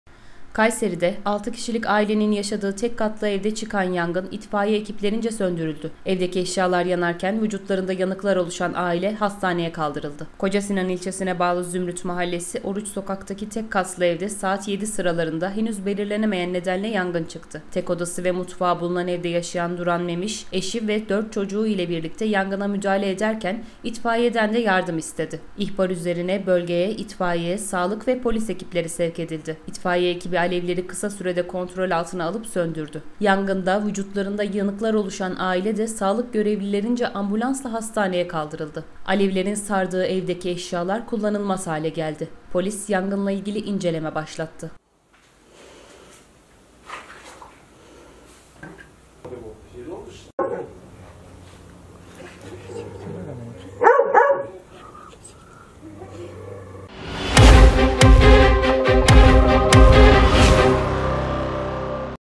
6 Kişilik Ailenin Evi Kül Oldu...
KAYSERİ'de, 6 kişilik ailenin yaşadığı tek katlı evde çıkan yangın, itfaiye ekiplerince söndürüldü. Evdeki eşyalar yanarken, vücutlarında yanıklar oluşan aile, hastaneye kaldırıldı.

Kocasinan ilçesine bağlı Zümrüt Mahallesi Oruç Sokak'taki tek katlı evde, saat 07.00 sıralarında, henüz belirlenemeyen nedenle yangın çıktı. Tek odası ve mutfağı bulunan evde yaşayan Duran Memiş, eşi ve 4 çocuğu ile birlikte yangına müdahale ederken, itfaiyeden de yardım istedi. İhbar üzerine bölgeye itfaiye, sağlık ve polis ekipleri sevk edildi. İtfaiye ekibi, alevleri kısa sürede kontrol altına alıp, söndürdü. Yangında vücutlarında yanıklar oluşan aile de sağlık görevlilerince ambulanslarla hastaneye kaldırıldı. Alevlerin sardığı evdeki eşyalar kullanılamaz hale geldi. Polis, yangınla ilgili inceleme başlattı.